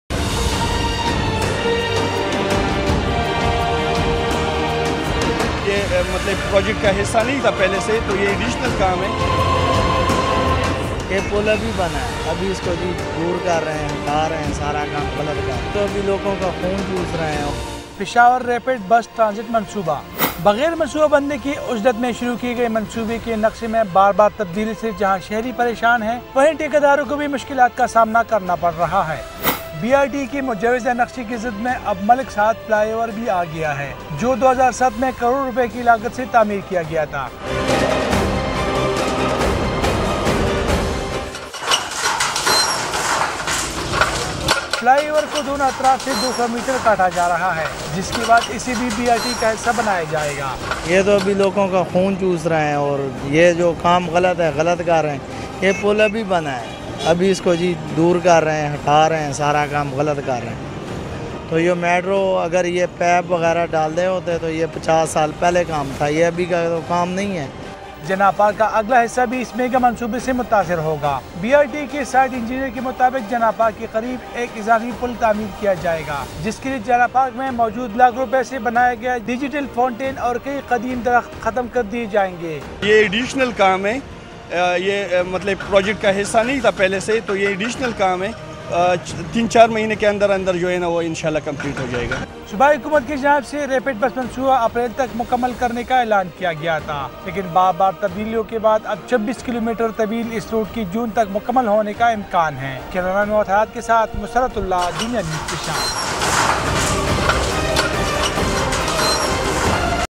new bridge for BRT
new bridge will construct for BRT in peshawar